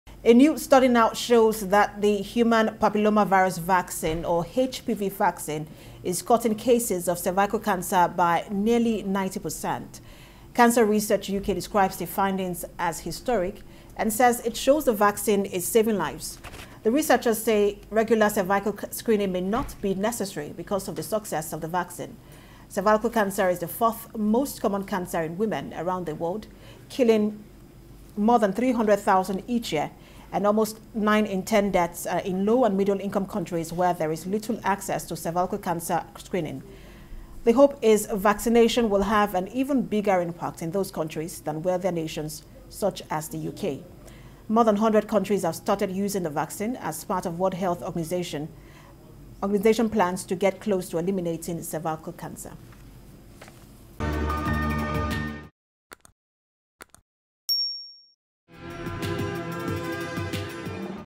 [LATEST] New Study Shows HPV Vaccine Is Cutting Cervical Cancer By Nearly 90%
A new study now shows that “the human papillomavirus vaccine, or HPV vaccine, is cutting cases of cervical cancer by nearly 90%”

Cancer Research UK describes the findings as "historic", and says it shows the vaccine is saving lives.